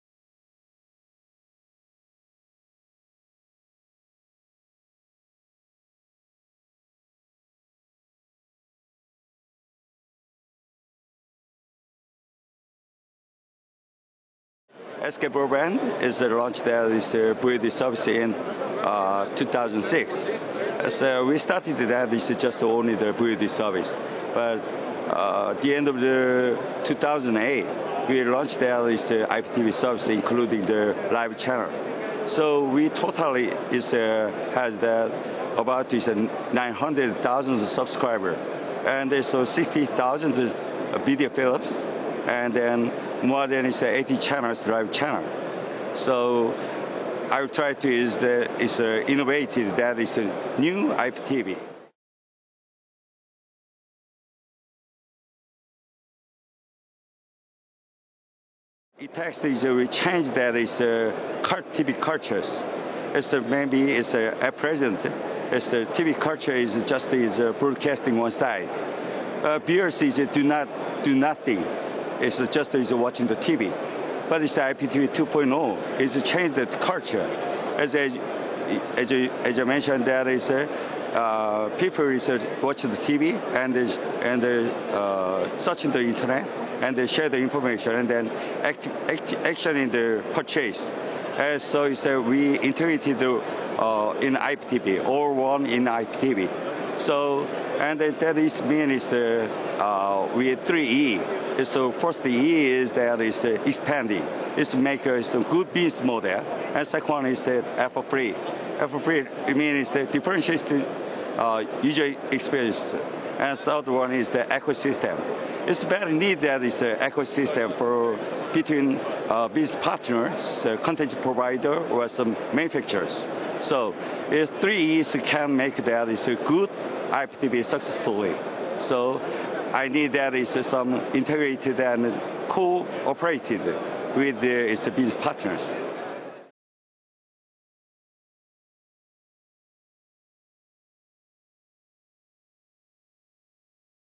JS Lee, President & CEO, SK Broadband
We speak to JS Lee, President & CEO of Korean telco SK Broadband at the IPTV World Forum 2010 event